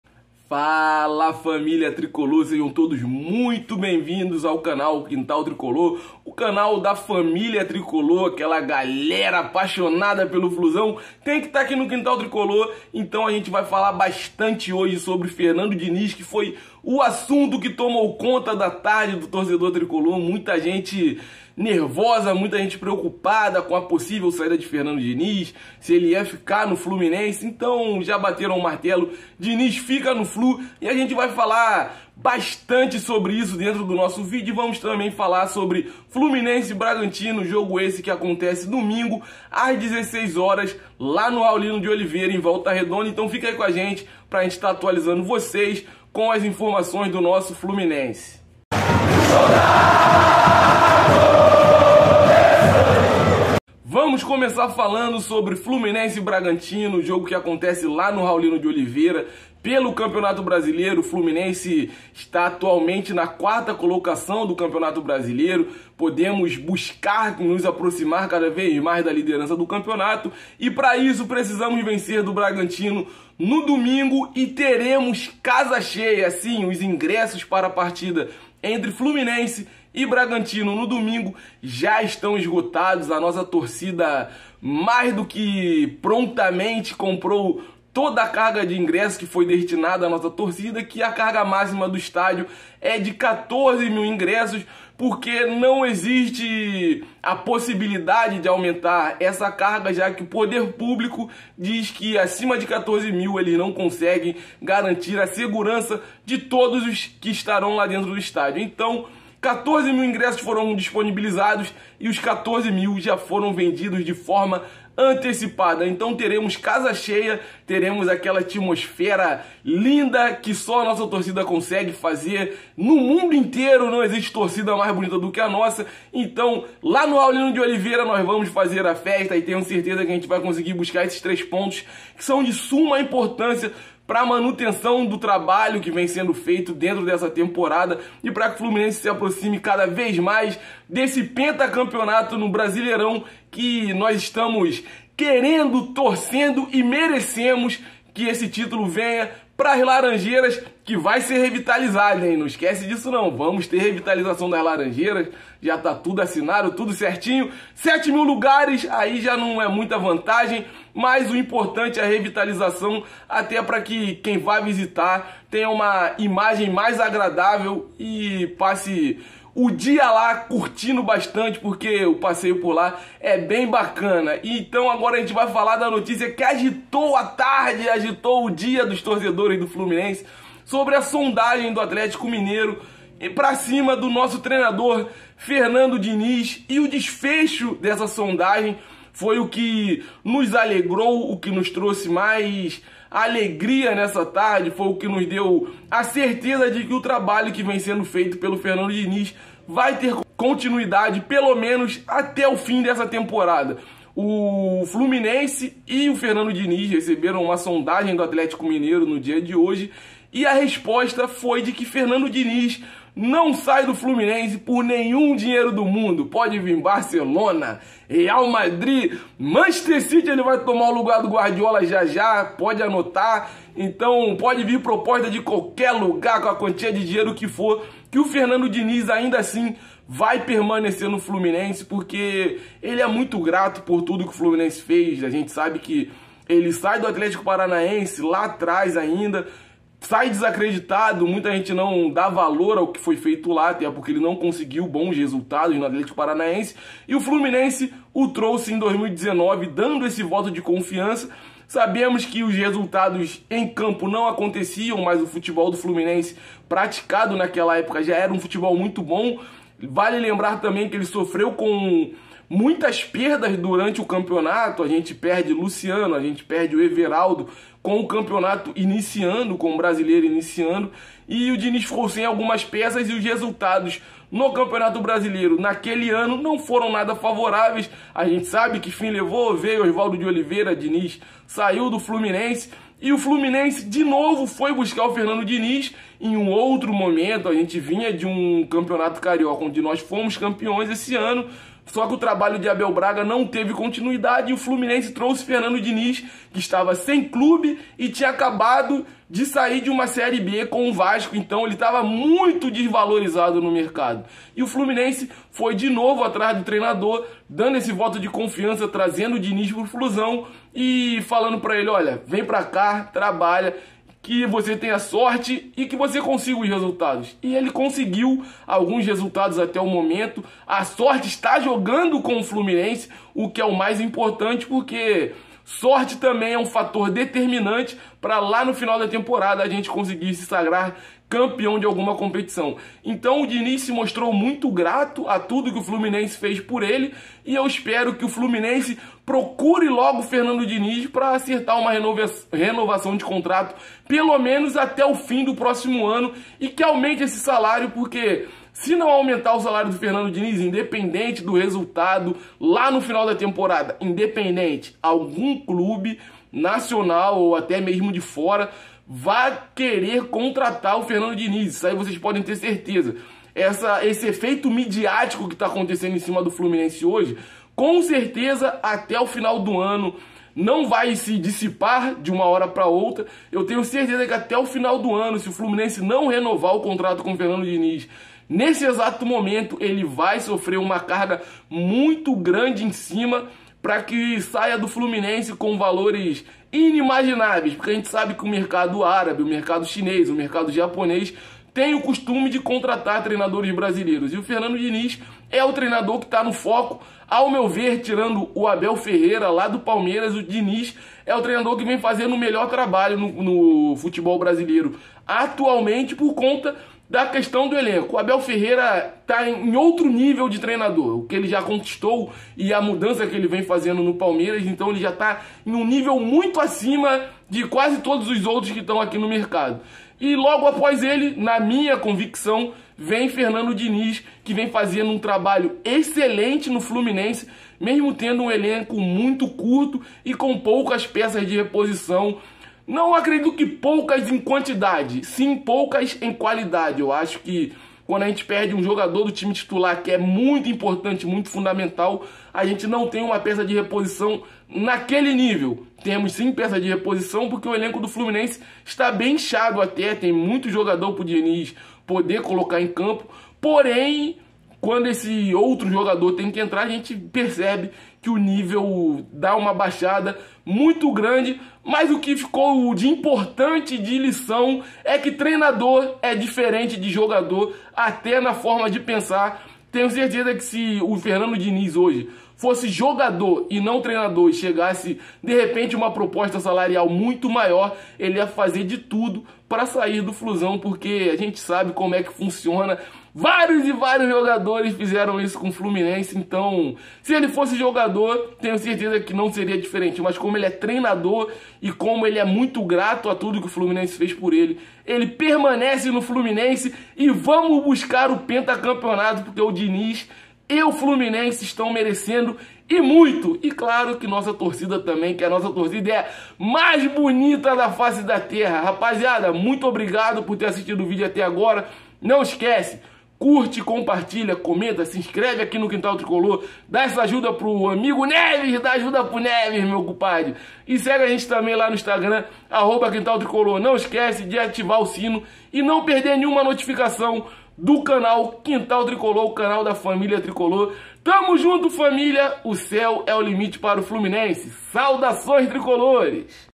FERNANDO DINIZ FICA NO FLU - CASA CHEIA NO DOMINGO!
Após movimentar a tarde de todos os tricolores, o técnico Fernando Diniz permanece no Fluminense e o sentimento de gratidão faz com que ele recuse proposta do Atlético-MG.
Ingressos esgotados de forma antecipada para a partida entre Fluminense e Bragantino pelo Brasileirão.

Saudações tricolores 🇭🇺